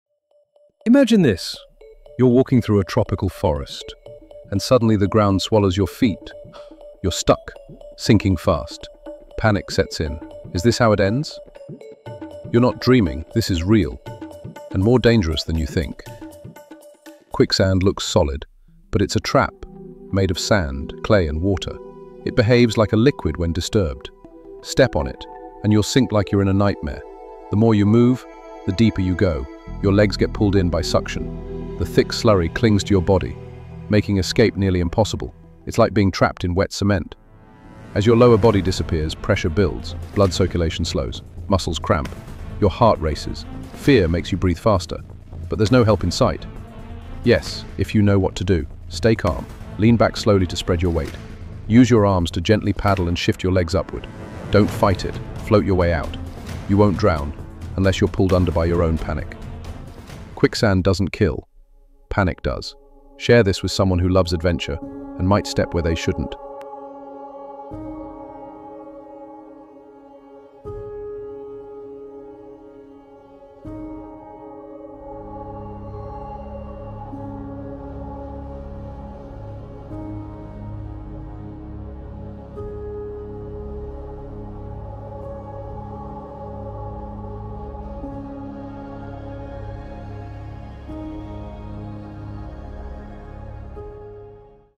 What Happens When You Fall into Quicksand?
He thought it was just dirt… until the earth swallowed him whole.

In this 2-minute cinematic survival breakdown, discover the terrifying truth about quicksand — what it is, how it pulls you in, and what your body goes through as you sink. Can you really survive it?

⚠️ Warning: This could save your life.

🎙️ Narrated by ElevenLabs (Adam Stone)
🎥 Visuals created with Hailuo AI